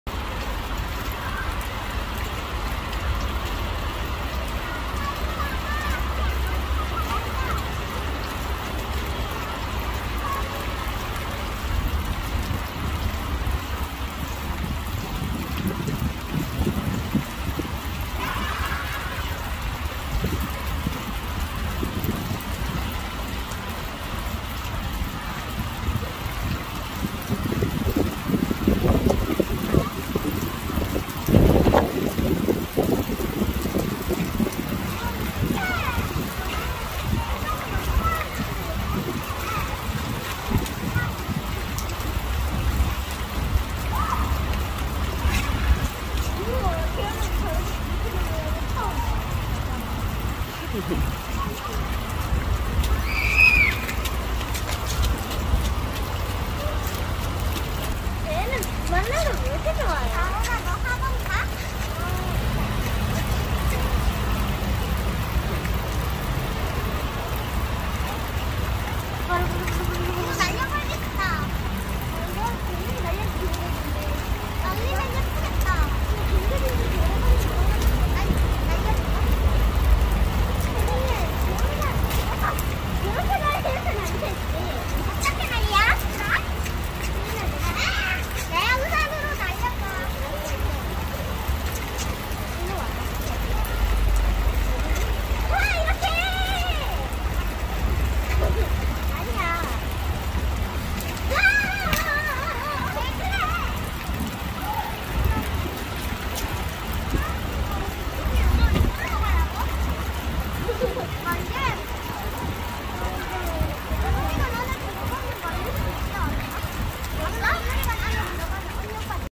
2. 임재우 학교 하원하고, 운동장에서 ☔️ 놀고 있는 영상 입니다. @BS재우스월드 [Jae Woo World]
2. 임재우 학교 하원하고, 운동장에서 ☔️ 놀고 있는 영상 입니다.
@BS재우스월드 [Jae Woo World]
ㆍ
왜 비오는데 그냥 보고 만 있었냐고 물어 보시는 분도 계실꺼에요.
재우가
"엄마 나 비오는 날 ☔️ 비를 한번 맞으면서 놀아보고 싶어 그럼 옷이 엉망이 되는데 그래도 괜찮아 ?"
난
"  어 그래 재우야 니가 하고 싶은데로 해봐."
라고 말했어요.
그래서 자연스럽게 영상을 찰영했습니다.^^
ㆍ
" 재우야 비를 맞으니까 기분이 어때 ?"
물었습니다.
" 마음이 편하고 기분이 좋았어.
비오는 날에 꼭 한번은 비를 맞으면서 놀이터에서 놀고 싶었어 "
라고 저에게 말했습니다.
집에와서
샤워하고, 잠시 놀다가
방금 학원으로 갔습니다.^^
ㆍ